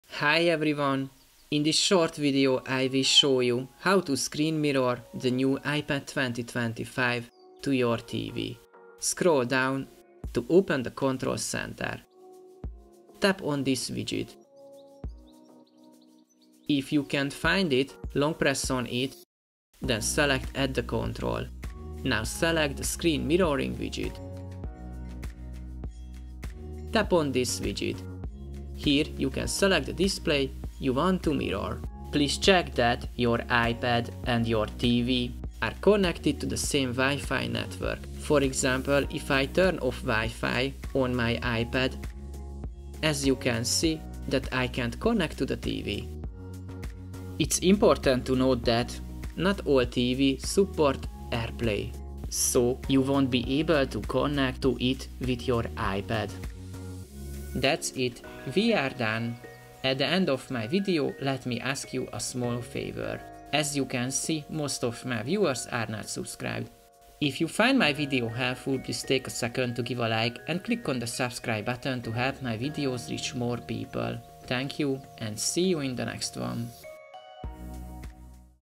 How To Screen Mirror iPad 11th Generation To TV
The How To Screen Mirror iPad 11th Generation To TV video finally arrived to the channel!

00:00 - Intro
00:09 - How to screen mirror

I hope you will enjoy this iPad 11 screen mirror to tv and I hope you will learn something new.